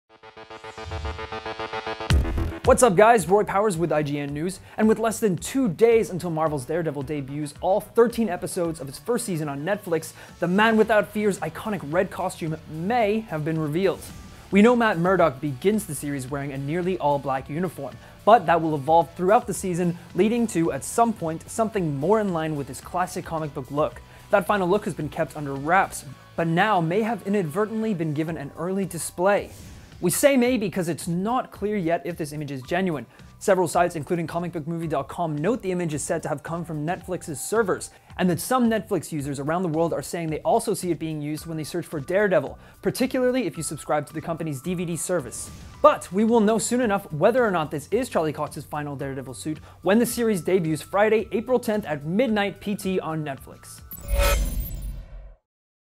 Daredevil's Red Costume May Have Been Revealed - IGN News
With less than two days until Marvel's Daredevil debuts all thirteen episodes of its first season on Netflix, the Man Without Fear's iconic red costume may have been revealed.